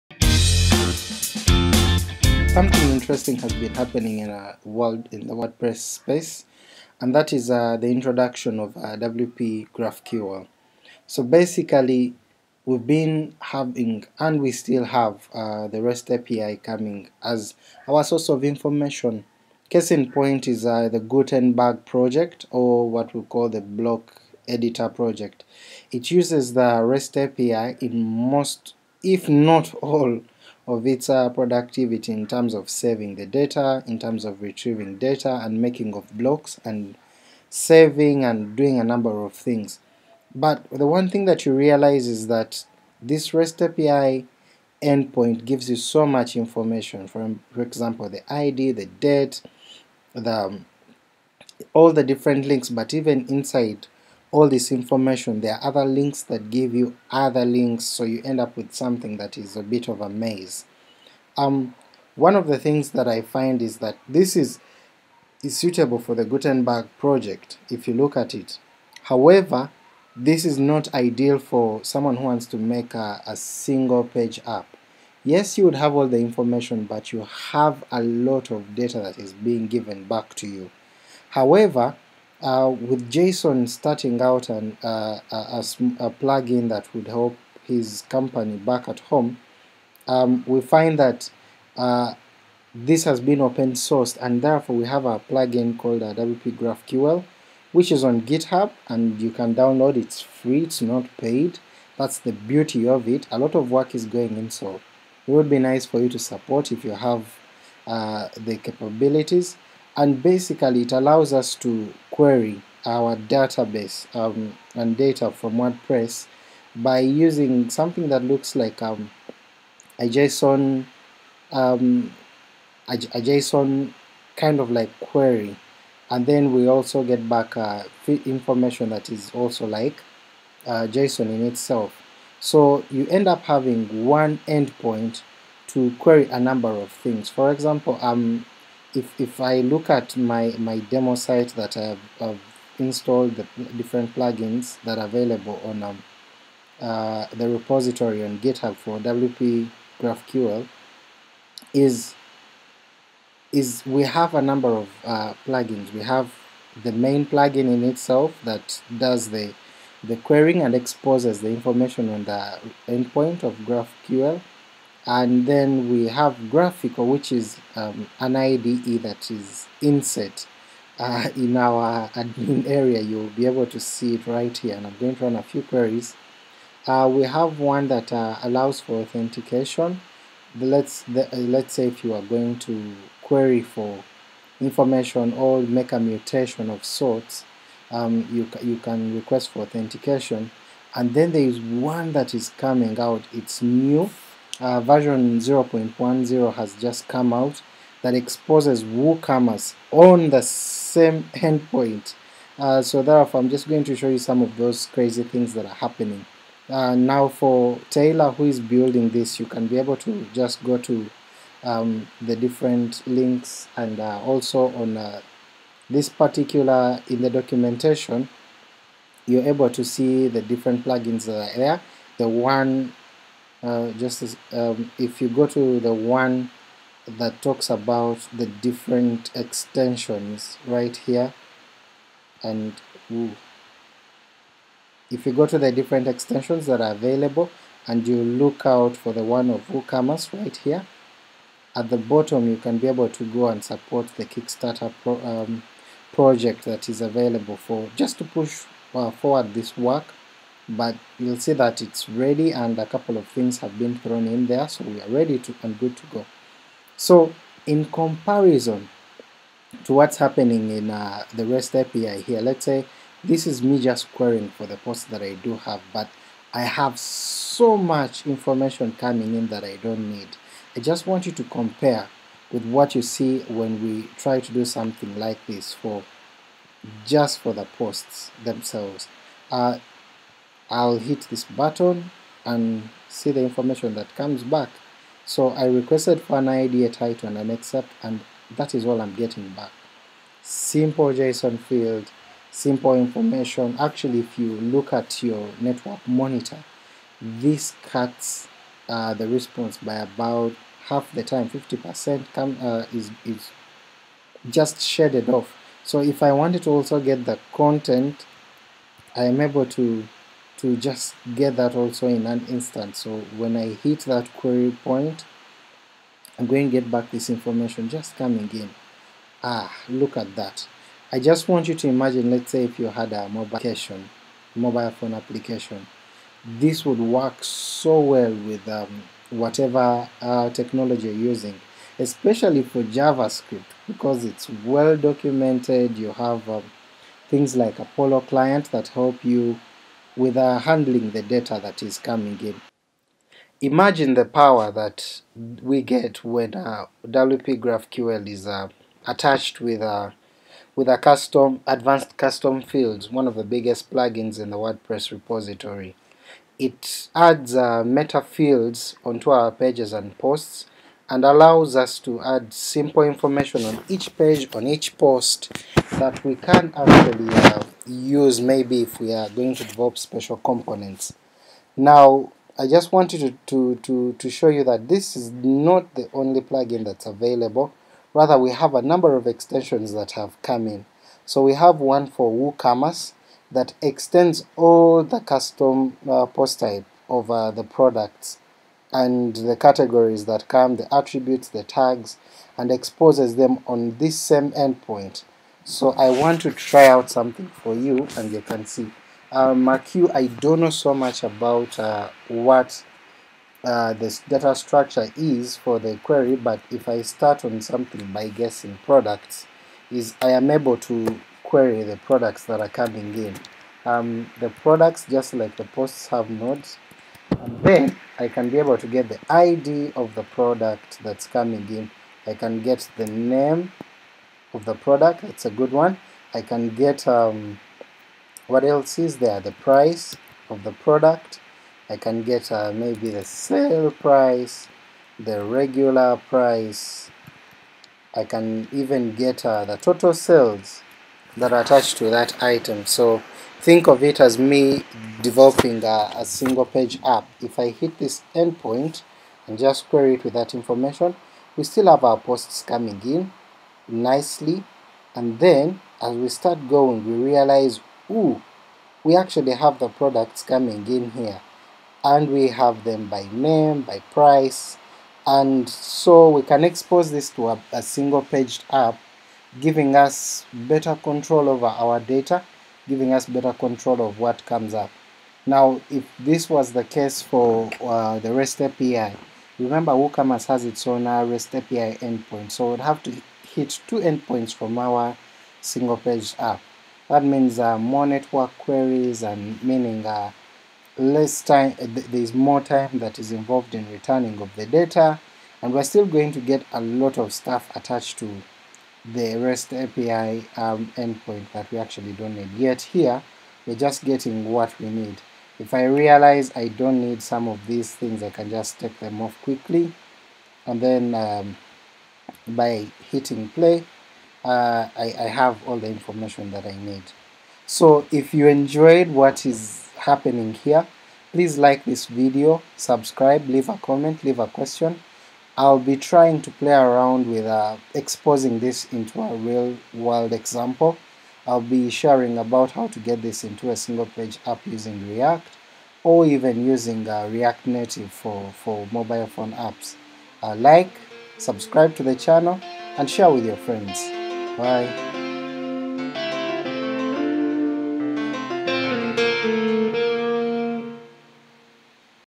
Using QraphQL in WordPress & WooCommerce with ACF, WGraphQL & WooGraphQL - Overview
Learn how GraphQL can be leveraged in WordPress as a headless Content Management System (CMS) using Advanced Custom Fields and WooCommerce plugins.

This is probably one of the greatest data handling jumps of WordPress, ACF and WooCommerce plugins since the WordPress REST API was introduced in the WordPress core.

With the start of WPGraphQL as a test, WordPress has been tested to make it a more versatile headless CMS for custom app and software builders. With the simplicity of WordPress admin side and the power of the GraphQL spec, we are yet to see more plugins adopt this approach and make info GraphQL ready. Maybe WordPress one day can look to having the spec as part of the core using the WPGraphQL plugin as a start point.